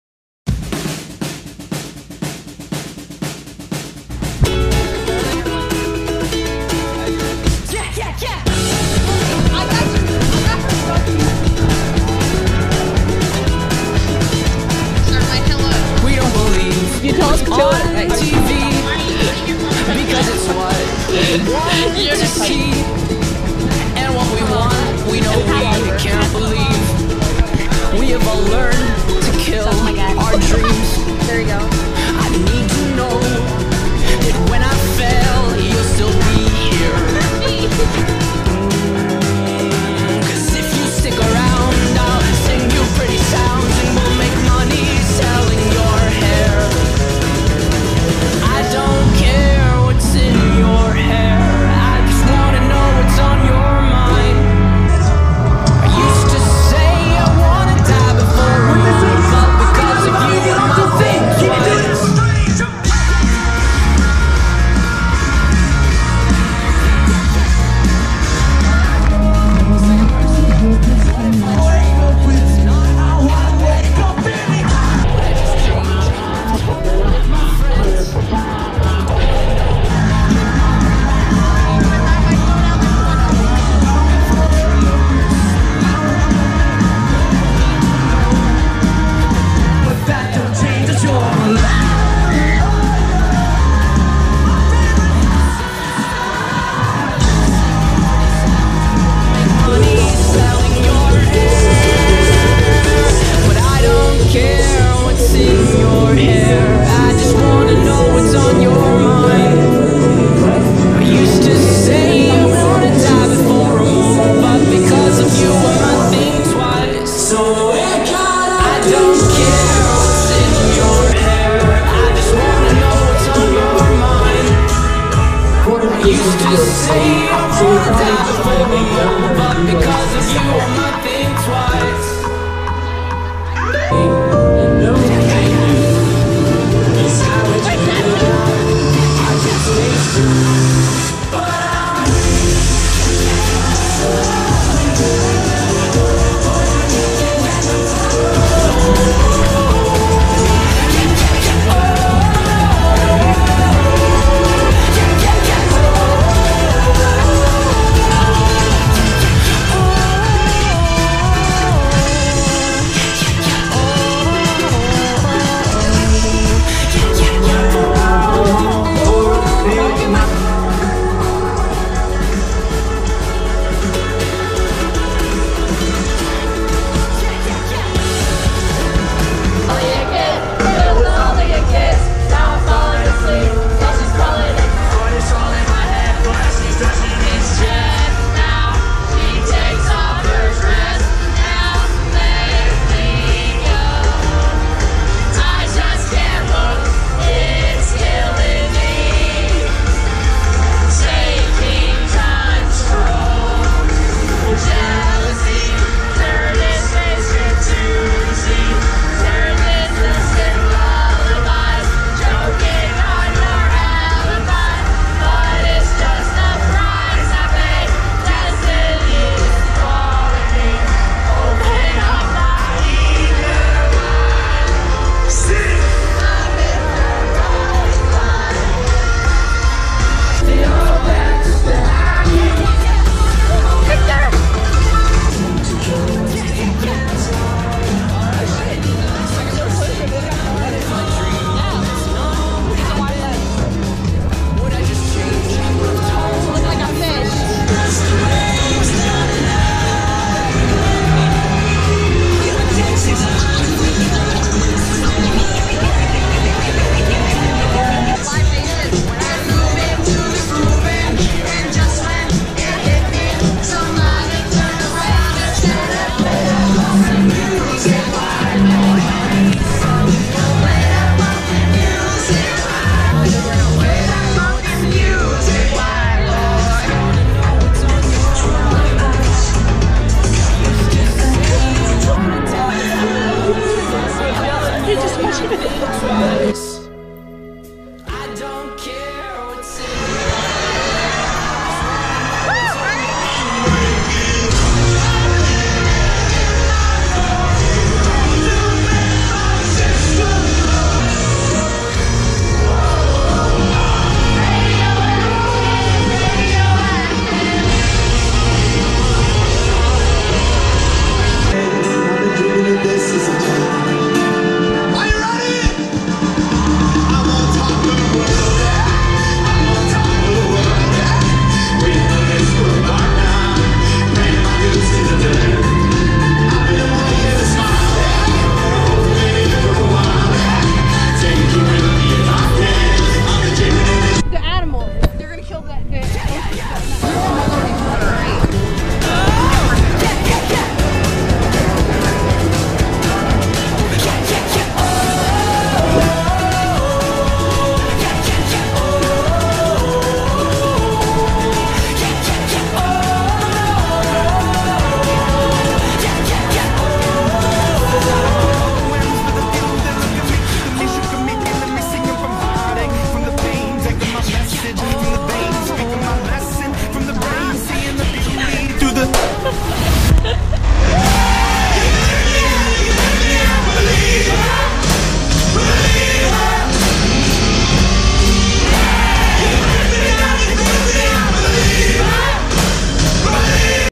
104.5 Birthday Show BB&T Pavillion 2018 GoPro Hero6
We went to a concert @ BB&T, it got interesting...

Thanks to Si, Grace, and G for allowing me to annoy them all day

104.5 Birthday Show

We took a ferry to Camden, NJ

We seen 'The Wrecks', Imagine Dragons, and AJR

Oh, and ate loads of Crabfries and drank from some Souvenir Cups

June 2018

Camden, NJ

Turn my notifications on to keep up with all of the travel vlogs!1!

VSCO: @sylwiazarzycka